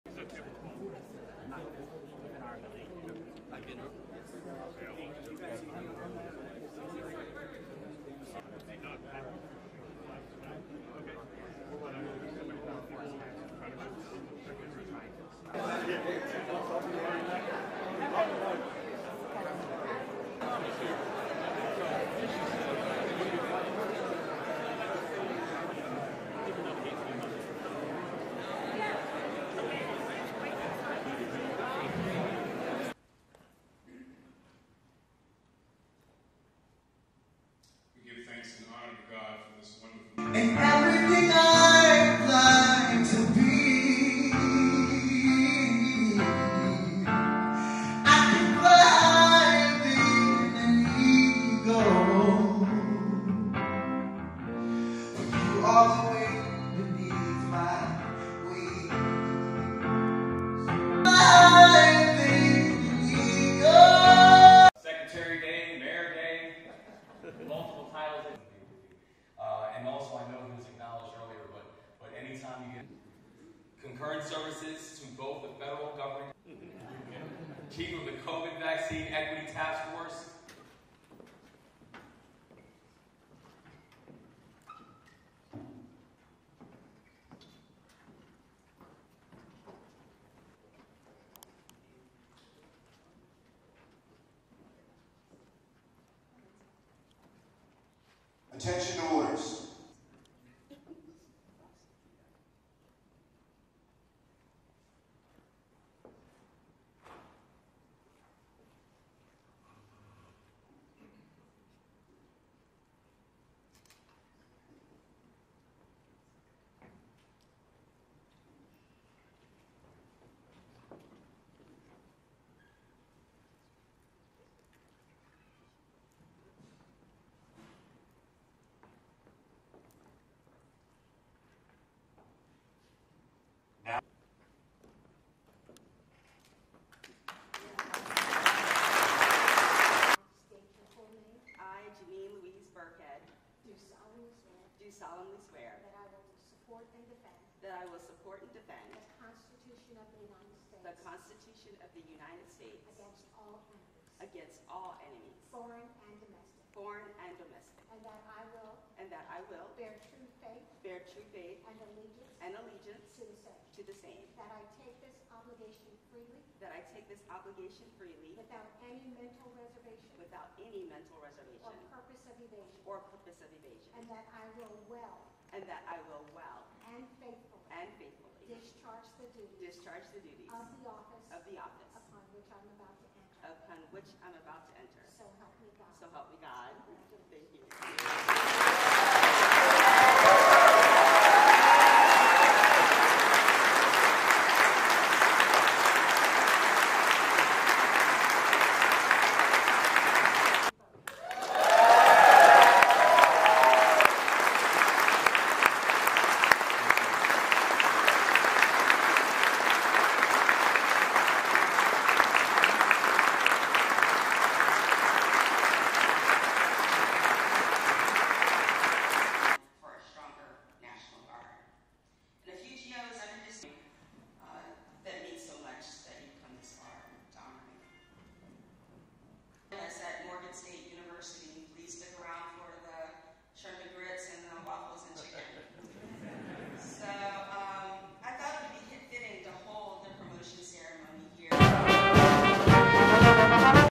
Maryland Military Department's promotion ceremony: A day to remember
Adjutant general promotion ceremony
BALTIMORE, MD, UNITED STATES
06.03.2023

 As adjutant general of Maryland, Birckhead serves on the governor’s cabinet and leads the Maryland Military Department, which includes the Maryland Army National Guard, Maryland Air National Guard and Maryland Defense Force. (U.S. Air National Guard photo by Senior Airman Rachel Underwood)(This photo has been cropped and color corrected to enhance the subject)

Film Credits: Video by Senior Airman Rachel Underwood
175th WG - Maryland Air National Guard